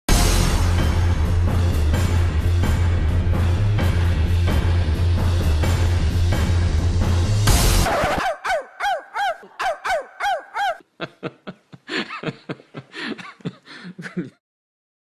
New Donald Trump ad against Clinton barking like a dog or bitch 3-16-2016
Hillary Clinton barking like a dog. Female dog or bitch. Putin is shown laughing at her. Is this what we want as president? Commercial release date: March 16, 2016 on Instagram